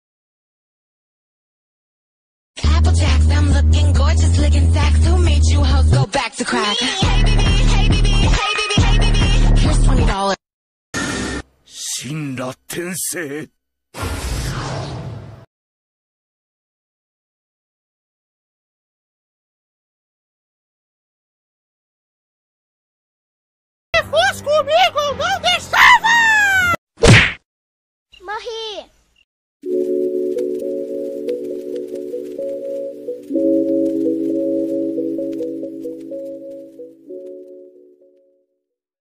Me ajuda a fazer ciúmes no Pain? (Meme Naruto) // Tobi Garanhão, Konan, Pain 💜 Gacha Club
Me ajuda a fazer ciúmes no Pain  Meme Gacha Club Naruto Shippuden Akatsuki
Tobi Garanhão